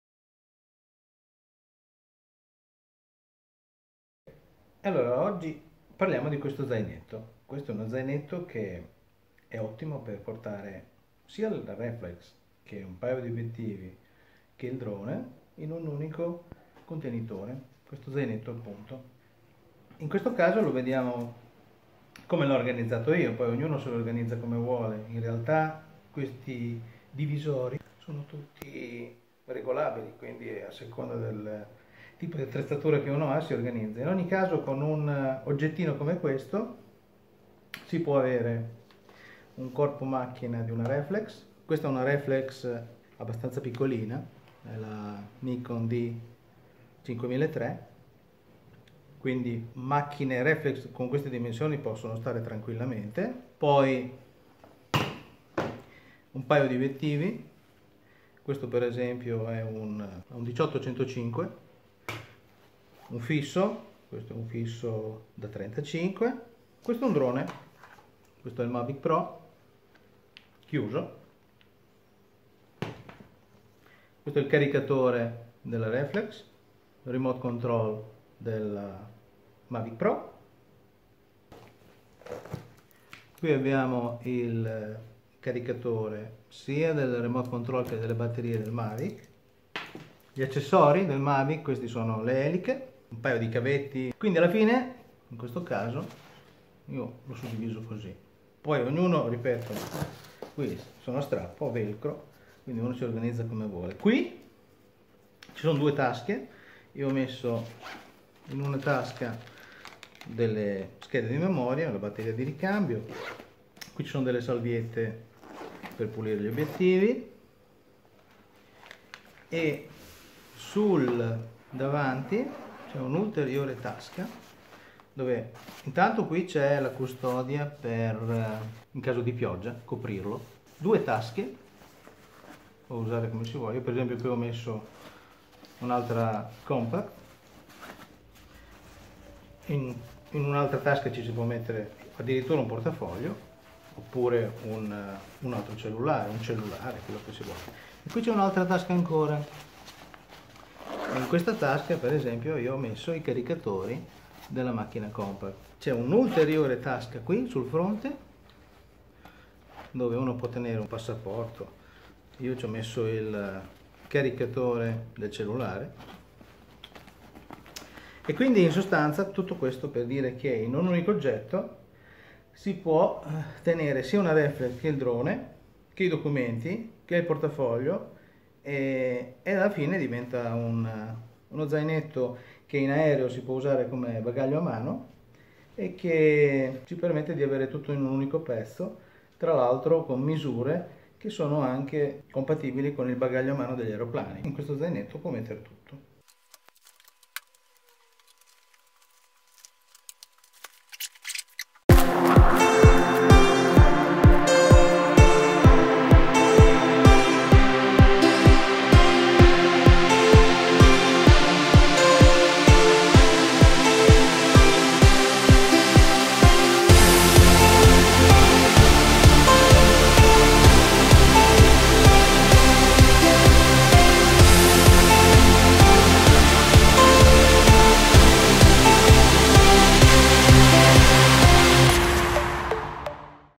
Zainetto Manfrotto all in one: reflex + obiettivi + drone + accessori & more
Zainetto manfrotto backpack manfrotto
Per reflex + drone + accessori e cellulare
Link con dettagli del modello:
Link with detailed info of the item: